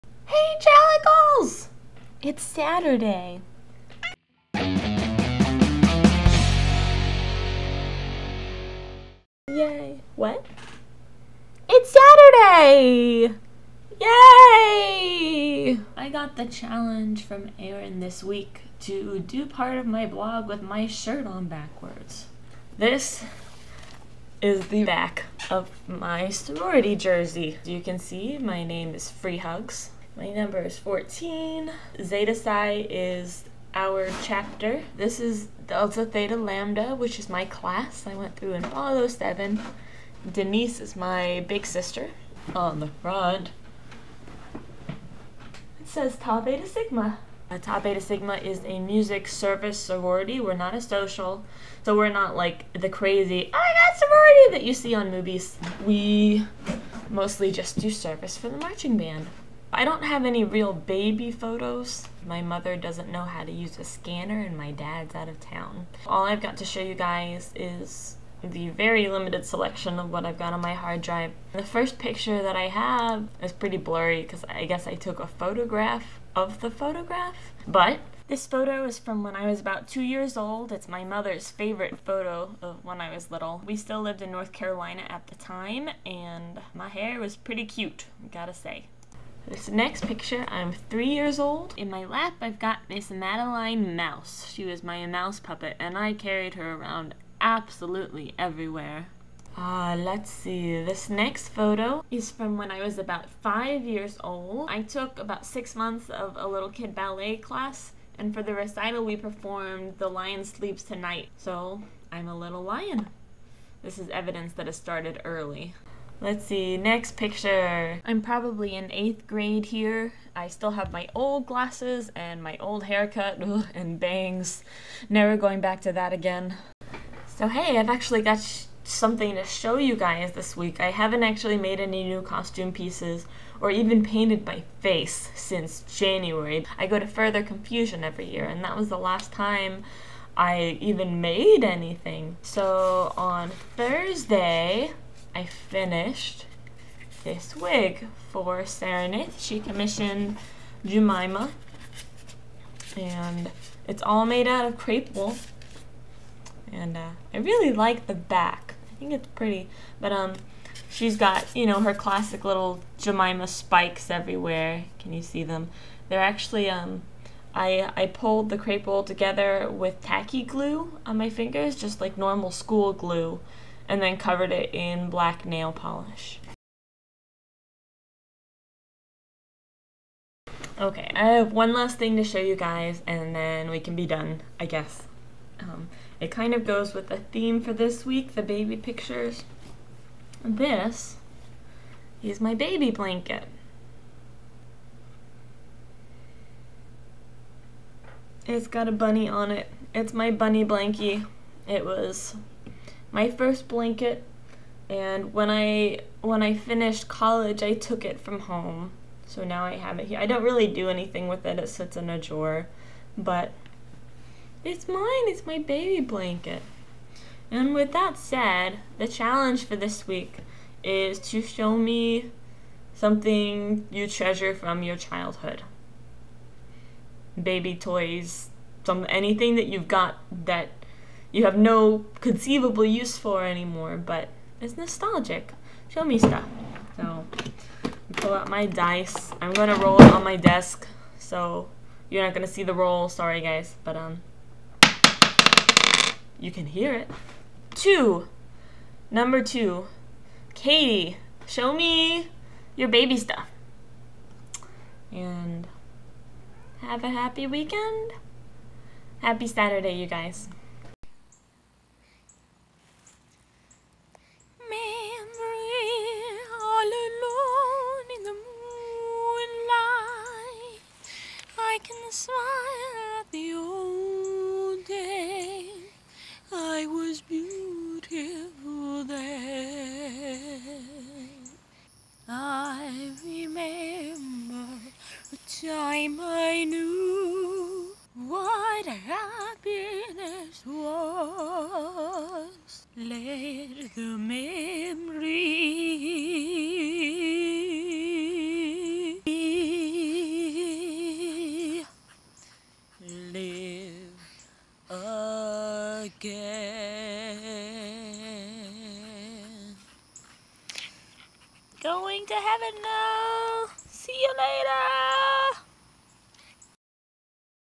In which old pictures come to light, wigs are made, and spoons sing
Question for the comments - do you guys know any salsa moves? Can you link me to a video of you or instructions? I've started going to a local salsa dance on Fridays and I want some more moves!